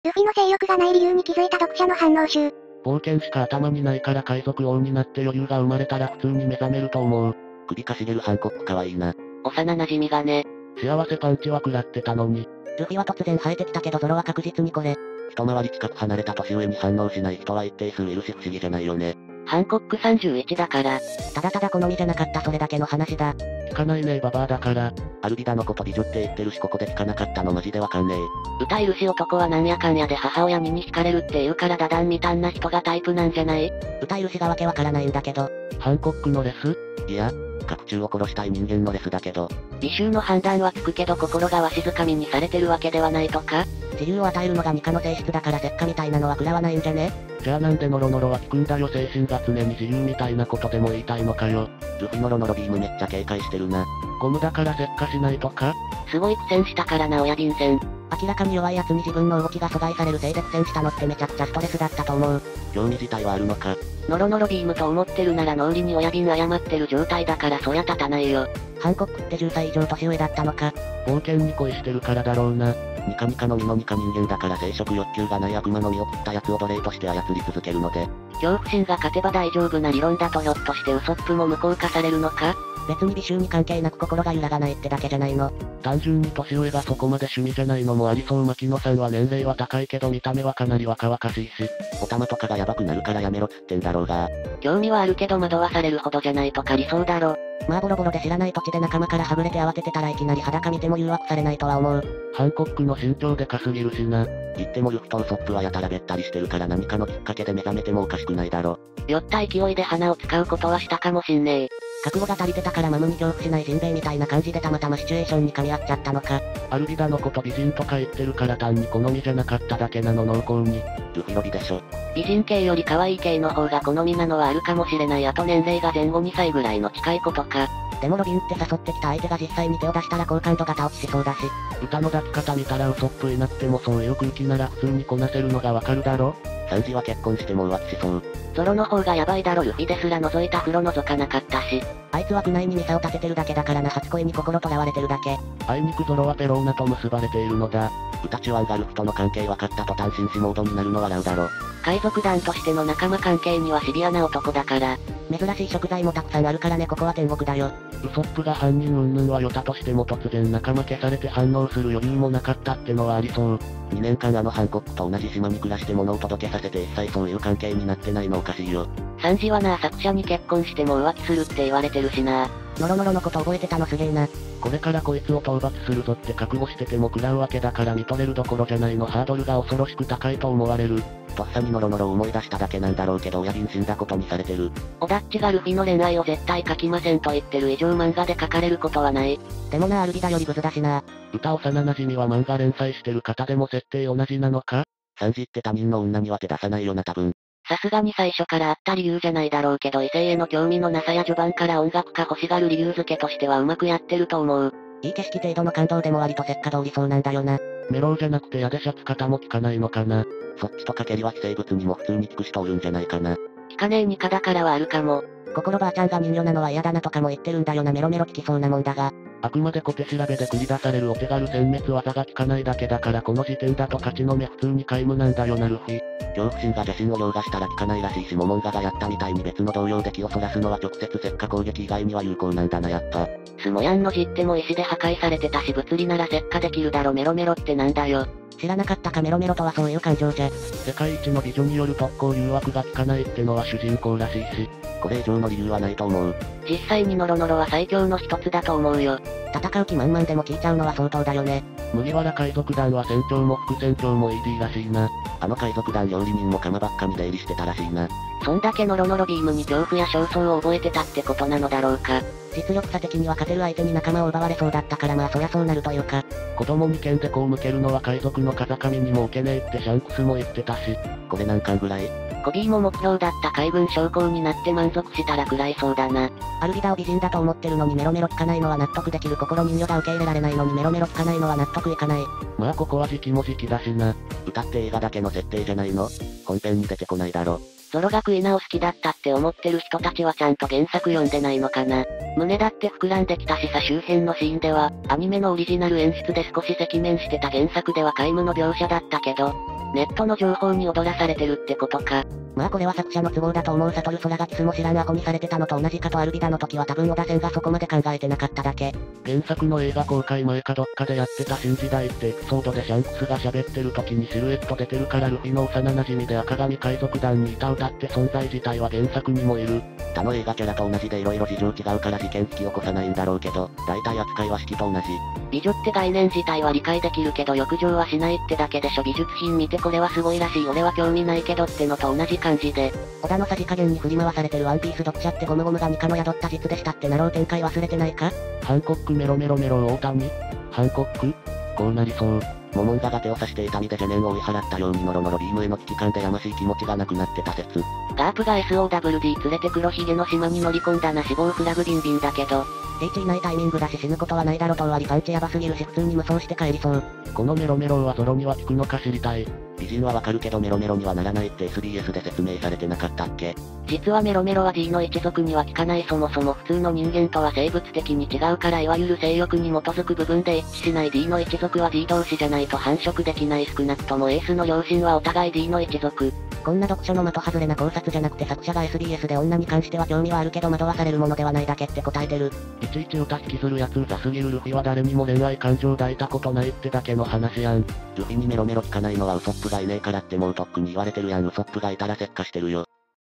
【ワンピース】ルフィに性欲がない理由に気づいた読者の反応集
このチャンネルでは、ワンピースに関するアニメ・漫画に対する読者の反応をまとめています！